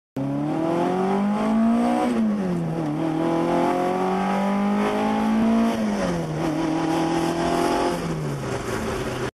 Alfa GTV 2L Engine Sound
1967 Alfa GTV with 2L, aluminum flywheel, Weber 45s, and 2.25" Magnaflow exhaust.